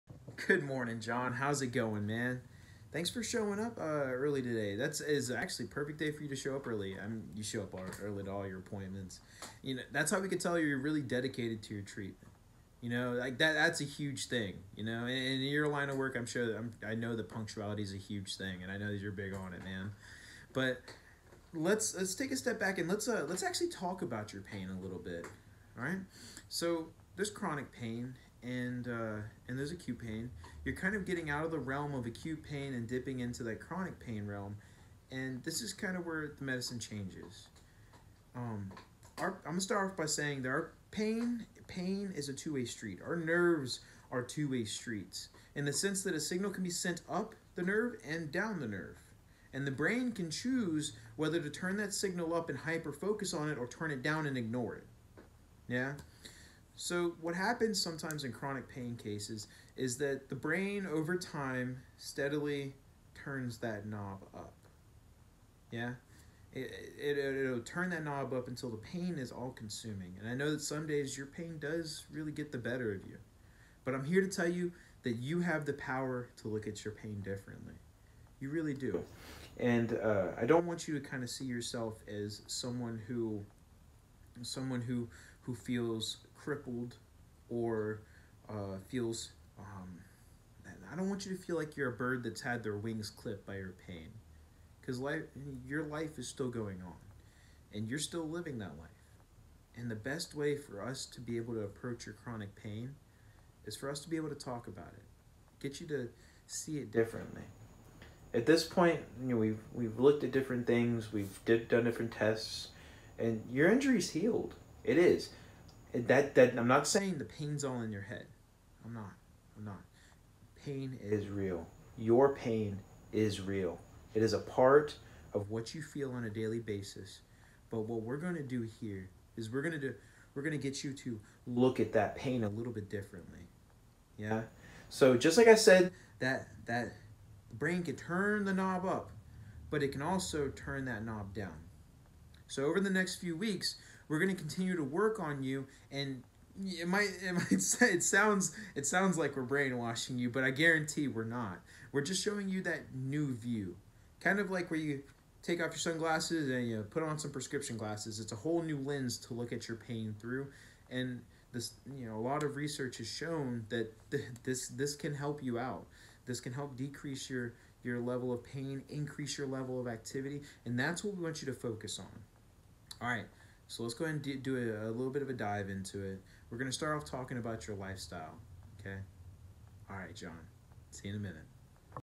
Pain Management Video Submission Part B
This is a conversation with a hypothetical patient for Arcadia University’s Hybrid DPT program.